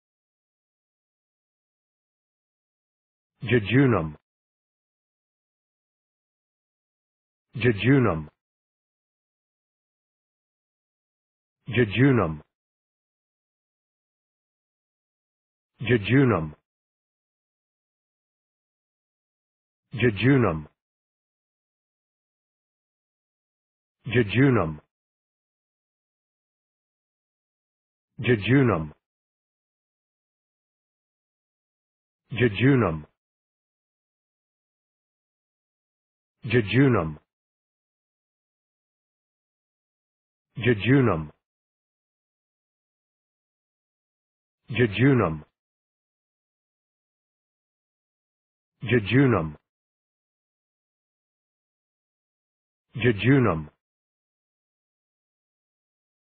Pronunciation of Jejunum | Definition of Jejunum
Jejunum pronunciation | How to pronounce Jejunum in English?
/dʒɪ`dʒuːnəm/
Meaning of Jejunum | What is Jejunum?
(1) (n.) The middle division of the small intestine, between the duodenum and ileum; `-`- so called because usually found empty after death.
What is Jejunum | Jejunum meaning and definition?